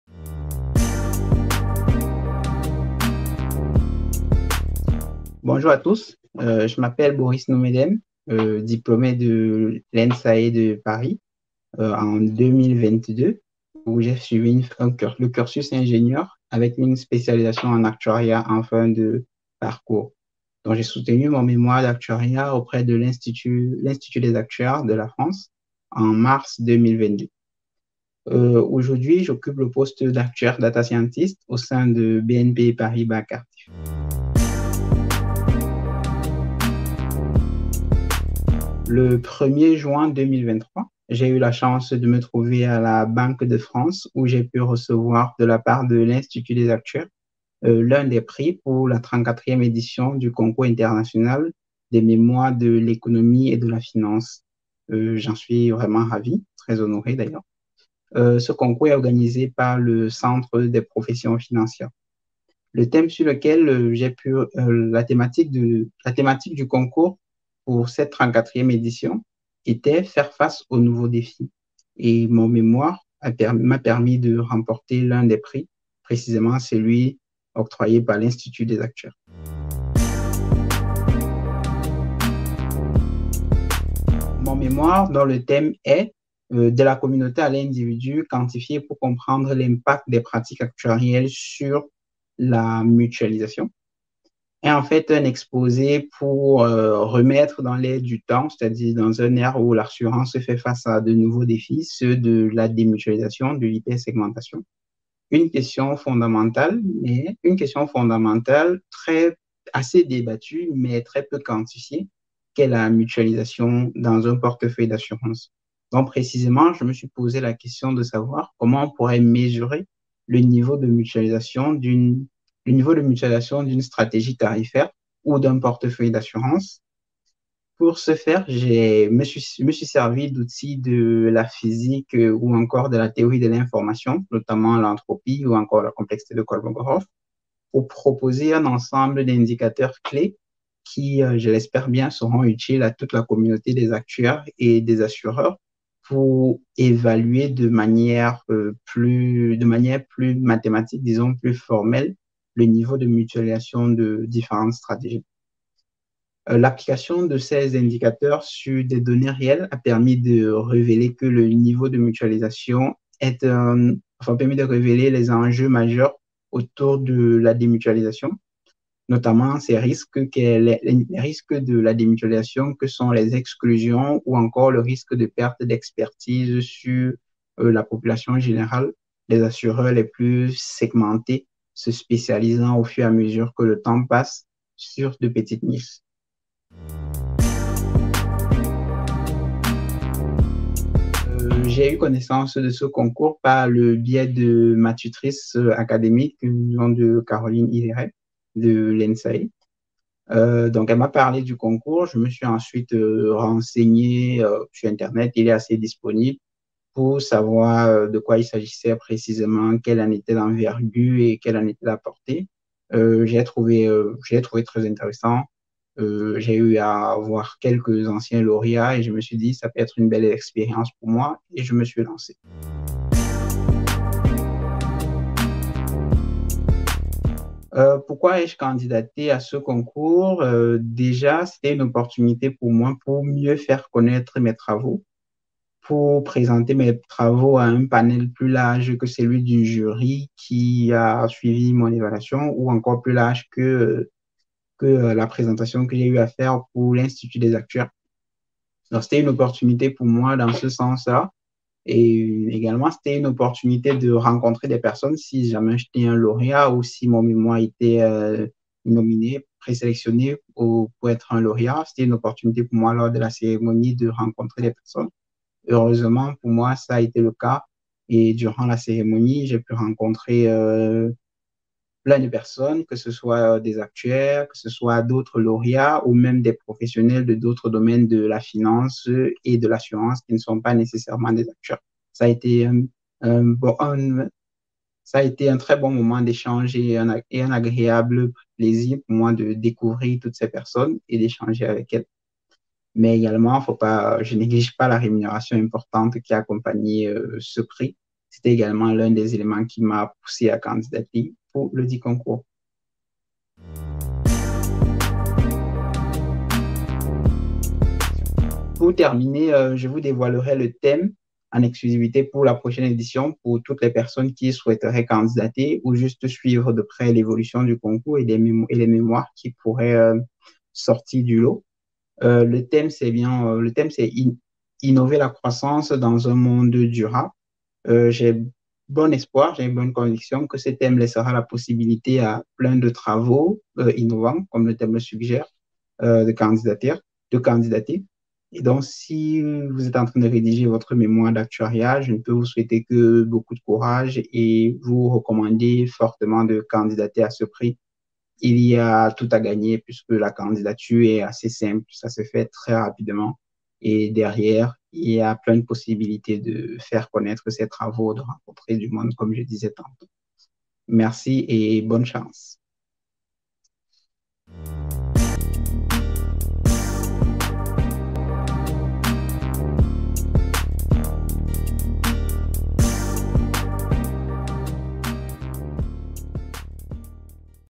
Interview Boris NOUMEDEM
Présentation de Boris NOUMEDEM lauréat du Prix "Institut des actuaires" du 34ème Concours International des Mémoires de l'Économie et de la Finance - édition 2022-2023. Le Prix "Institut des actuaires" a été remis à Boris NOUMEDEM par Philippe TALLEUX, Vice-Président de l'Institut des actuaires, le jeudi 01 juin 2023 à la Banque de France pour son mémoire "De la communauté à l’individu : Quantifier pour comprendre l’impact des pratiques actuarielles sur la mutualisation".

Retrouvez l'intégralité du mémoire de Paul-Emmanuel sur le site du Centre des Professions Financières